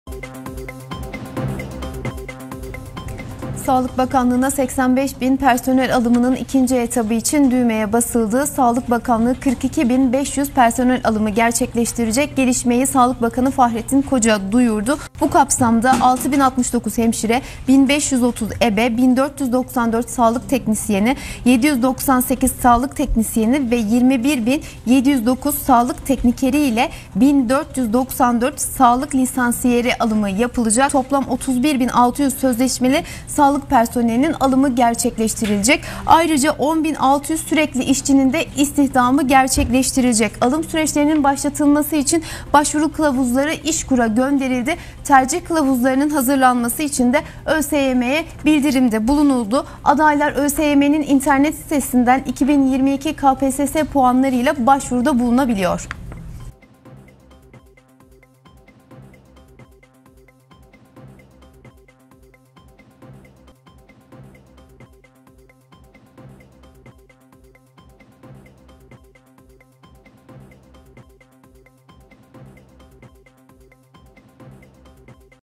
Sağlık Bakanlığı 42 bin 500 personel alımı 2023
Sağlık Bakanlığı 42 bin 500 personel alımı 2023: Sağlık Bakanlığı 2. etap personel alımı başladı mı, ne zaman yapılacak? ÖSYM KPSS ve İŞKUR başvuru ve tercih kılavuzu ne zaman yayımlanacak? İşte kadro ve branş bilgisi...

Sağlık Bakanlığı, 2. etap 42 bin 500 personel alımıyla ilgili Bakan Fahrettin Koca'dan açıklama geldi. Bakan Koca açıklamasında, alım süreçlerinin başlatılması için, başvuru kılavuzlarının İŞKUR'a ve tercih kılavuzunun hazırlanmak üzere ÖSYM'ye gönderildiğini ifade etti. Ayrıca alım yapılacak branşları da paylaştı.

Personel alım süreciyle ilgili olarak bilgileri paylaşan Bakan Koca, şu ifadelere yer verdi: "KPSS ile 6.069 hemşire, 1.530 ebe, 1.494 sağlık lisansiyeri, 798 sağlık teknisyeni ve 21.709 sağlık teknikeri olmak üzere, toplam 31.600 sözleşmeli sağlık personeli ve ayrıca 10.900 sürekli işçi istihdam edilecektir. Alım süreçlerinin başlatılması için, başvuru kılavuzları İŞKUR'a ve tercih kılavuzu hazırlanmak üzere ÖSYM'ye gönderilmiştir.